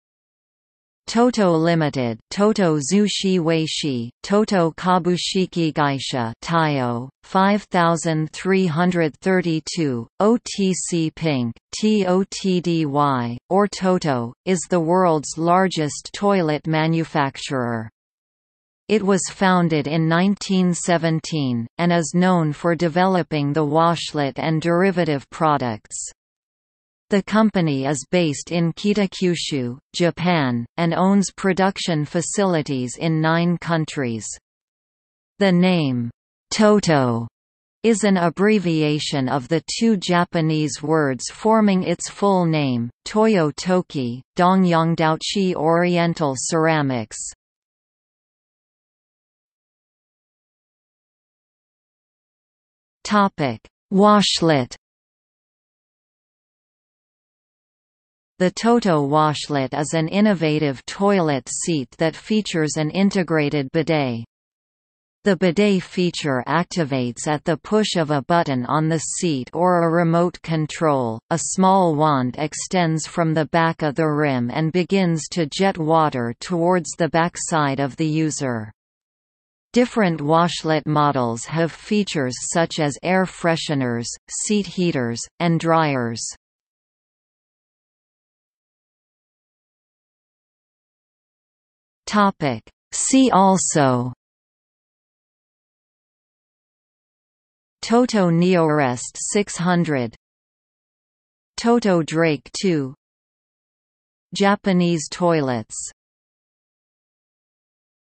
TOTO (company) | Wikipedia audio article
00:00:49 Washlet
00:01:28 See also

- Socrates

SUMMARY
Toto Ltd. (TOTO株式会社, Tōtō Kabushiki-gaisha) (TYO: 5332, OTC Pink: TOTDY), or TOTO, is the world's largest toilet manufacturer. It was founded in 1917, and is known for developing the Washlet and derivative products. The company is based in Kitakyushu, Japan, and owns production facilities in nine countries. The name "Toto" is an abbreviation of the two Japanese words forming its full name, Tōyō Tōki (東洋陶器, Oriental Ceramics).